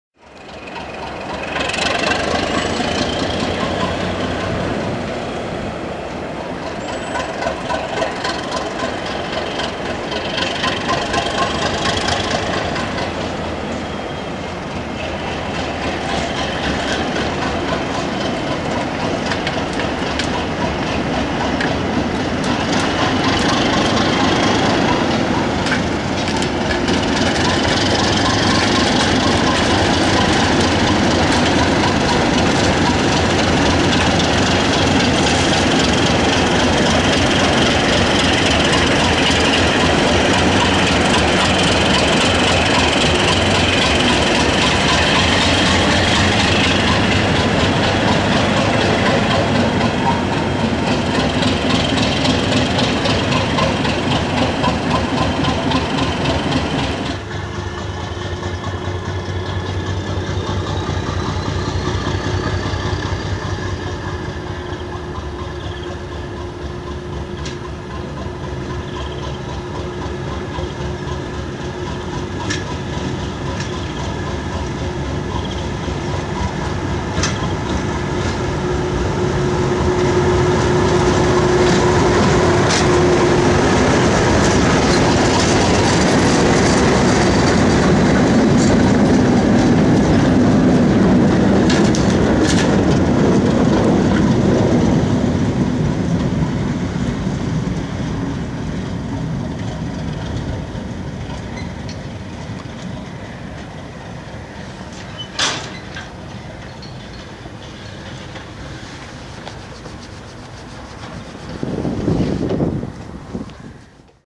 45132 towed by 37/9 D6836 at Ropley on 0R91 for Alresford (MHR 2013)
37905 and 45132 on 0R91 Ropley - Alresford light engine move at Ropley, 28/4/13.